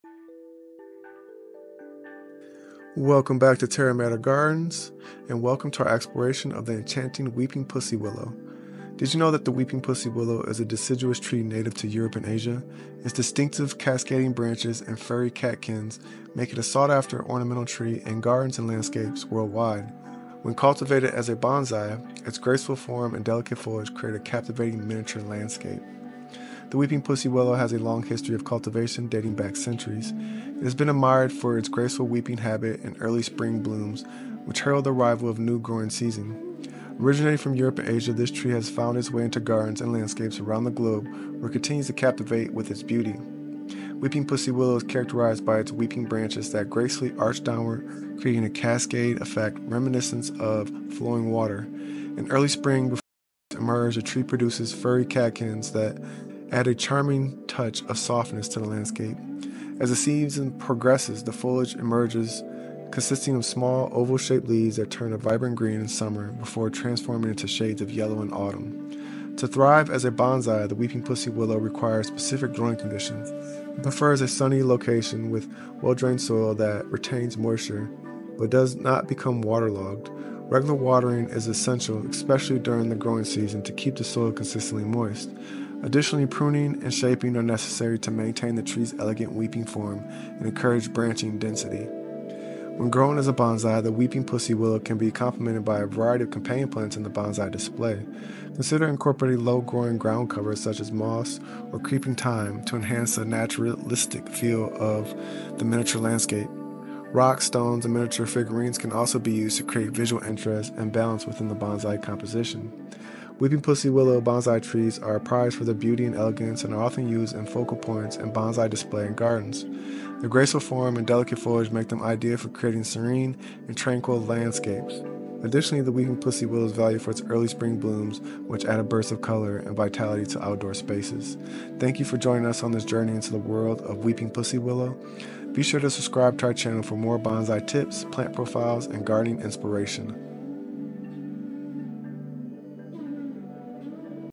Growing WEEPING PUSSY WILLOW: Quick Tips for (Salix caprea 'Pendula')
Explore essential tips for cultivating Weeping Pussy Willow, scientifically known as Salix caprea 'Pendula', in just 2 minutes! Learn about its growth requirements, pruning techniques, and care methods to ensure a graceful and picturesque addition to your landscape. Whether you're a seasoned gardener or new to tree cultivation, this concise guide provides valuable insights into growing Weeping Pussy Willow successfully. Don't miss out on this quick and informative overview of nurturing this elegant tree!

Latin Name: Salix caprea pendula
Common Name: Weeping Pussy Willow
Sun Exposure: Full sun to partial shade
Watering: Keep soil consistently moist, do not allow to dry out completely
Soil: Well-draining, loamy soil
Height: Typically 6-10 feet (1.8-3 meters) in bonsai form
Spread: Varies depending on the size of the bonsai
Foliage: Deciduous
Bloom Time: Spring
Flower Color: Yellow
Hardiness Zone: USDA zones 4-8
Maintenance: Moderate to high
Special Features: Graceful weeping habit, catkins in spring
Uses: Outdoor bonsai, ornamental specimen
Growing Tips: Prune regularly to maintain desired shape and size. Pinch back new growth to encourage branching. Provide regular fertilization during the growing season. Protect from strong winds and heavy snow loads in winter. Repot every 2-3 years in early spring before new growth appears.
Note: Weeping Pussy Willow bonsai are prized for their graceful cascading branches and early spring catkins. They require regular maintenance to keep their shape and health, but can be a rewarding addition to a bonsai collection.